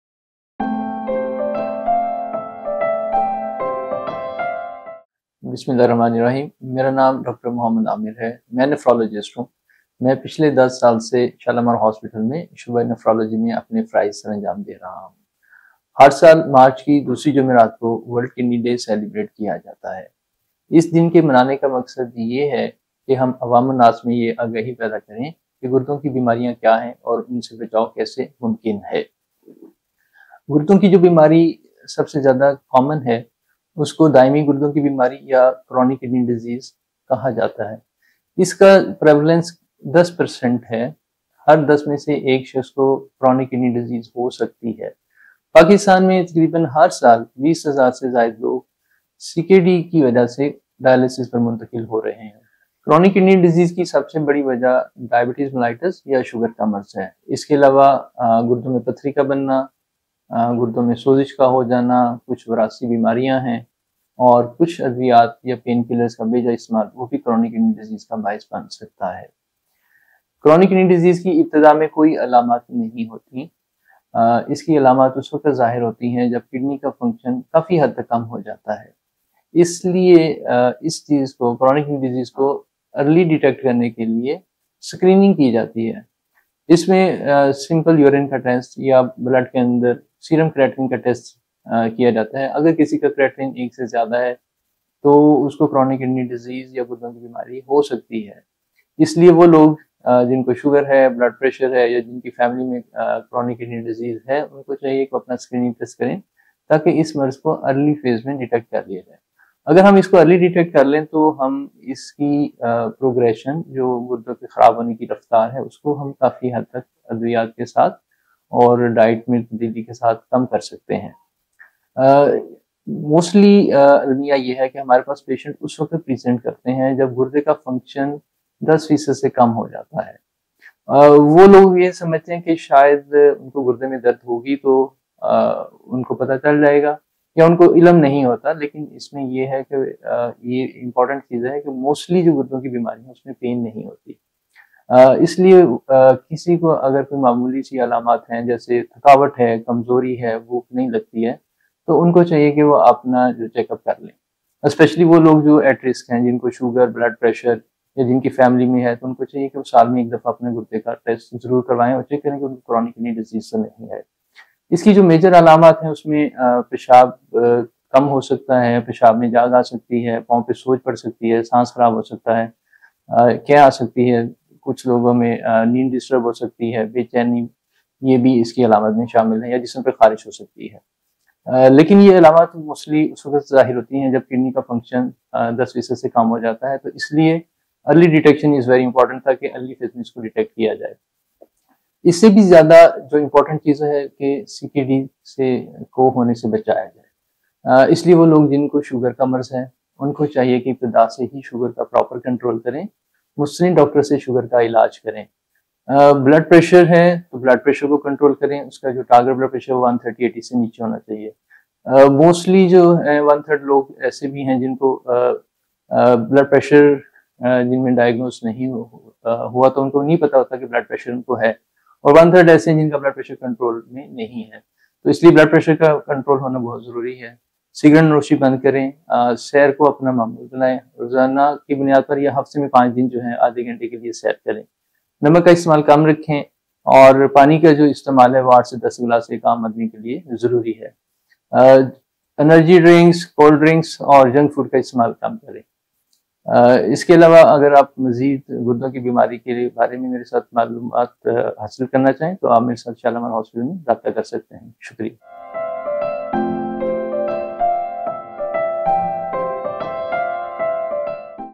Protect Your Kidneys: Watch This Important Message on World Kidney Day!
Dr. Muhammad Aamer, Head of Nephrology at Shalamar Hospital, shares a critical information on kidney diseases and their symptoms. Early detection is key!
Here's why you should watch:
Understand the importance of kidney health.
Learn about common kidney diseases and their warning signs.
Gain valuable insights from a leading nephrology expert.